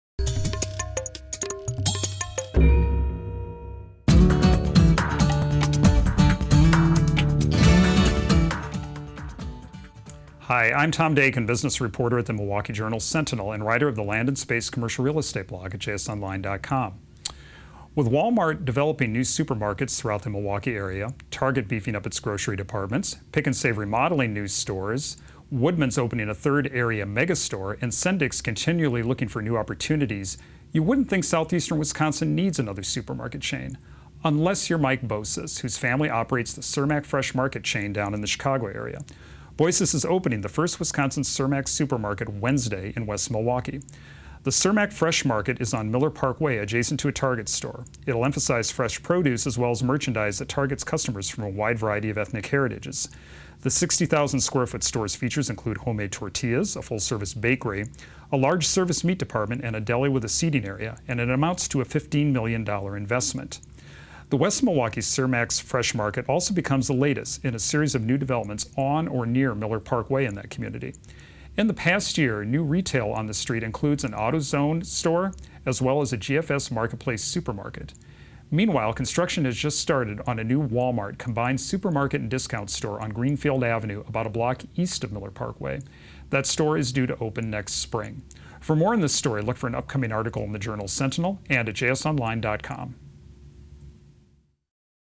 Land and Space: Cermak Fresh Market
Business reporter Tom Daykin talks about the new Cermak Fresh Market that is opening on Miller Park Way.